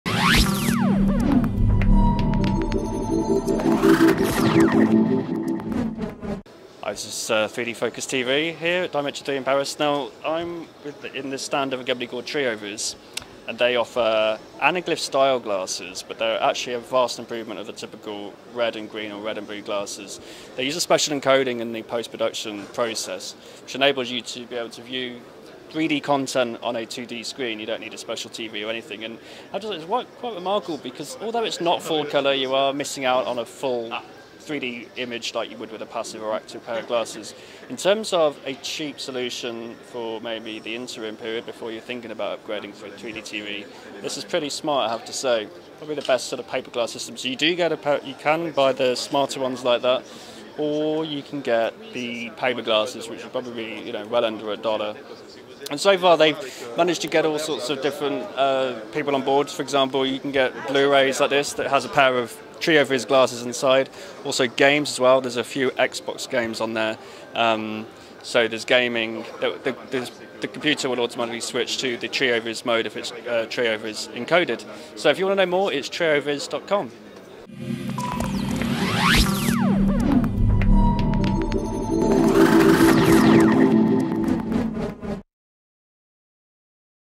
The Trioviz 3D System
During Dimension 3 in Paris I looked at the Trioviz 3D system and I was genuinly impressed. Of course, it's not as good as active or passive 3D but certainly the best coded system for 2D TVs. Trioviz DVDs and games are encoded in a way that delivers a 3D effect whilst minimalising the colour problems of coloured glasses. Very cheap effective bridge between 2D and 3D TVS.